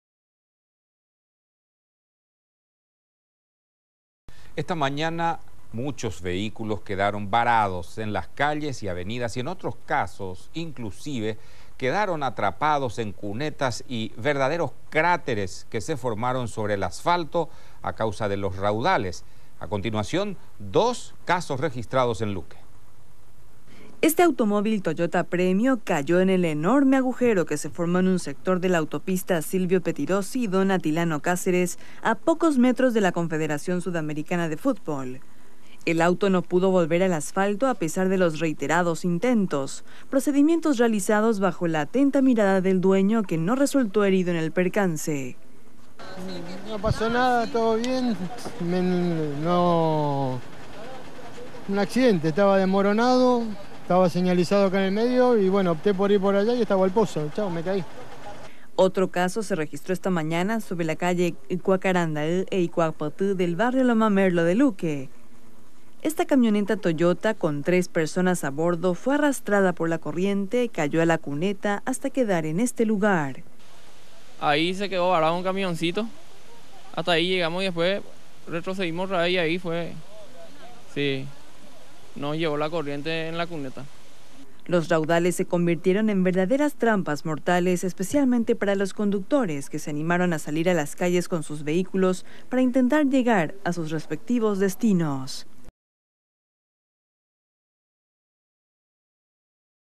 Cunetas y cráteres formaron las intensas lluvias - 26/02/2014
Esta mañana muchos vehículos quedaron varados en las calles y avenidas y en otros casos inclusive quedaron atrapados en cunetas y verdaderos cráteres que se formaron sobre el asfalto a causa de los raudales. A continuación 2 casos registrados en Luque.

Telediario